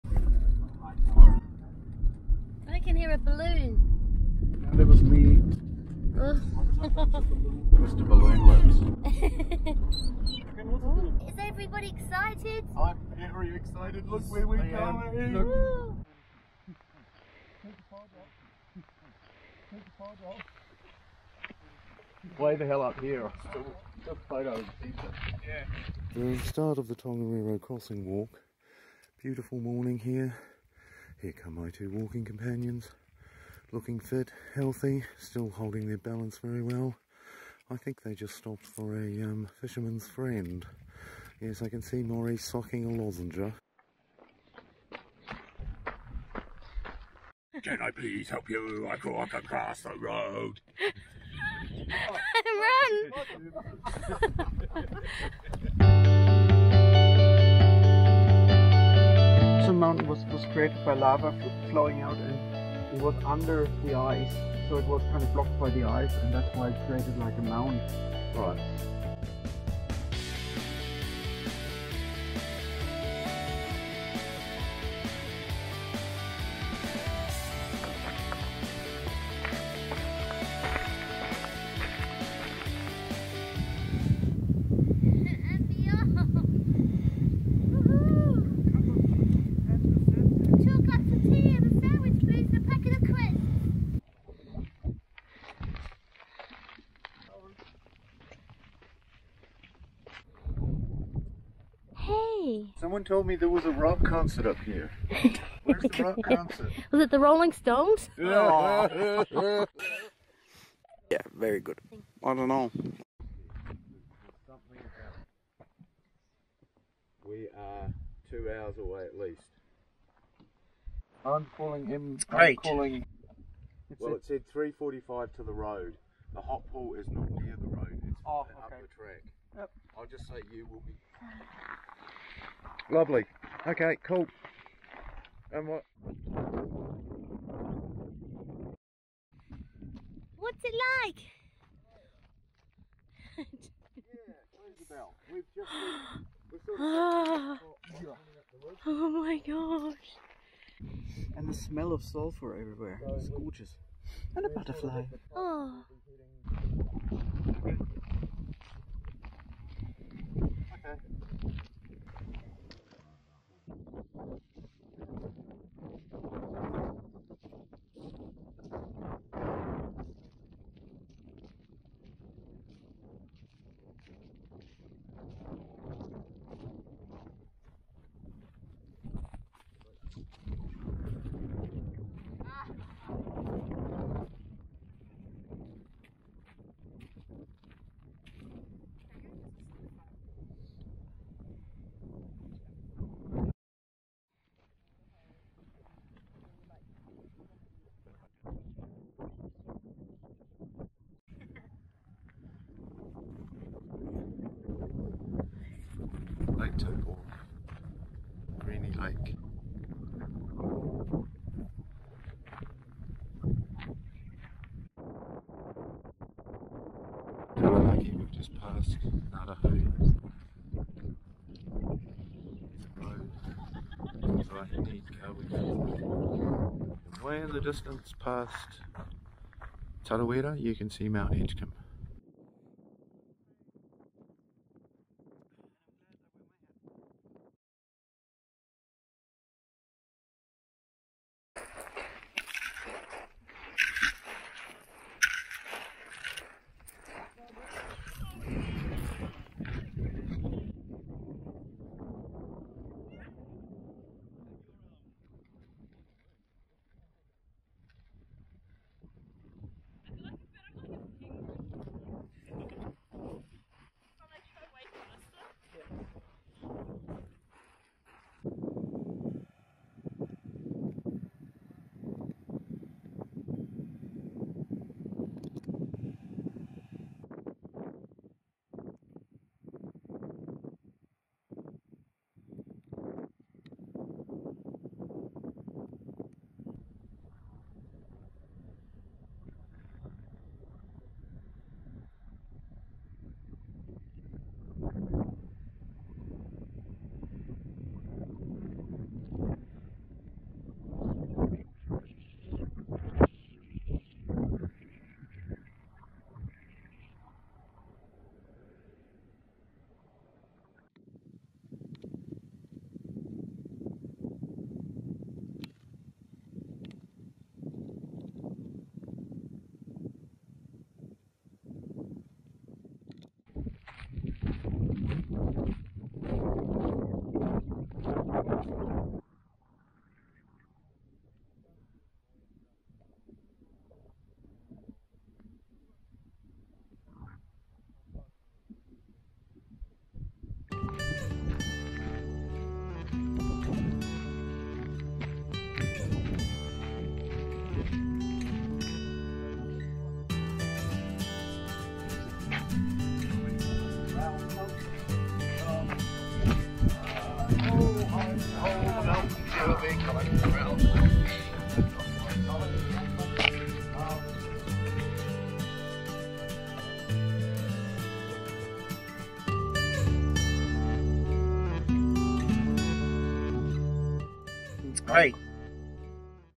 Tongariro Crossing Final
Here's the incredible Tongariro Crossing trek I did earlier in the year with John Ellis and Maurice Reviol. It took 8.5 hours but here I've condensed it to 6 mins. If you don't have time, jump to 2.5 mins to catch the epic summit and run with me down to the green and blue lakes. Spectacular!